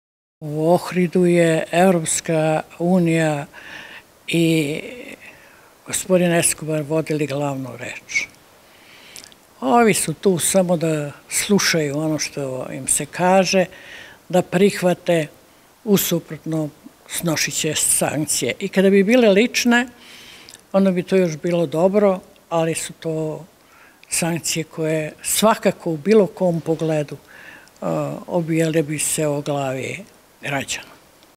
Rada Trajković – U Ohridu su EU i Eskobar vodili glavnu reč dok su ostali slušali šta im se kaže
Predsednik Srbije Aleksandar Vučić rekao je da u Ohridu ništa nije potpisao jer Kosovo nije međunarodno pravno priznata država sa kojom bi mogao da potpiše međunarodni sporazum. On je ponovio da su crvene linije za Srbiju – priznanje nezavisnosti Kosova i članstvo Kosova u UN, te da samo do tih crvenih linija Srbija može da radi na implementaciji dogovorenog. Osvrćući se na tekst aneksa, predsednik je kazao: „Ovo  za nas nije posebno teško. Biće nam mnogo težih stvari u budućnosti“. Kako komentariše sastanak u Ohridu pitali smo predsednicu Evropskog pokreta Srba sa Kosova i Metohije Radu Trajković.